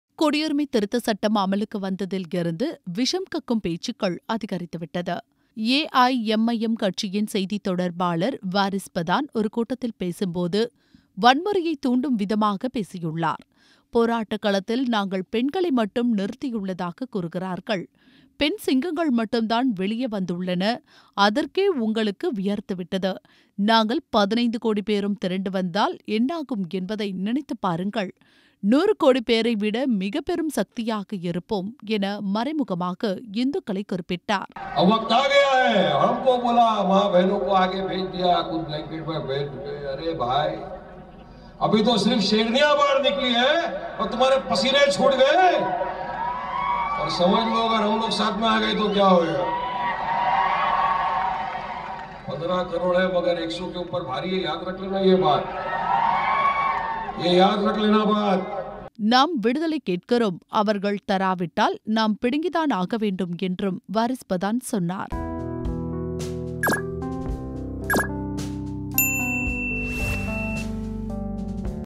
'15 கோடி முஸ்லிம் திரண்டு வந்தா 100 கோடி தாங்காது'
குடியுரிமை திருத்தச்சட்டம்
அமலுக்கு வந்ததில் இருந்து
விஷம் கக்கும் பேச்சுக்கள்
அதிகரித்துவிட்டது.

AIMIM  கட்சியின்
செய்தித் தொடர்பாளர்
வாரீஸ் பதான்
ஒரு கூட்டத்தில் பேசும்போது
வன்முறையை தூண்டும் விதமாக
பேசியுள்ளார்.

&#39;&#39;போராட்டக்களத்தில்
நாங்கள் பெண்களை மட்டும்
நிறுத்தியுள்ளதாக கூறுகிறார்கள்.
பெண் சிங்கங்கள்  மட்டும்தான்
வெளியே வந்துள்ளன.
அதற்கே உங்களுக்கு
வியர்த்து விட்டது;
நாங்கள் 15 கோடி பேரும்
திரண்டு வந்தால்
என்னாகும் என்பதை
நினைத்துப்பாருங்கள்.
100 கோடி பேரை விட
மிகப்பெரும் சக்தியாக இருப்போம்&#39;&#39;
என மறைமுகமாக
இந்துக்களை குறிப்பிட்டார்.

நாம் விடுதலை கேட்கிறோம்.
அவர்கள் தராவிட்டால்
நாம் பிடுங்கித்தான் ஆக வேண்டும்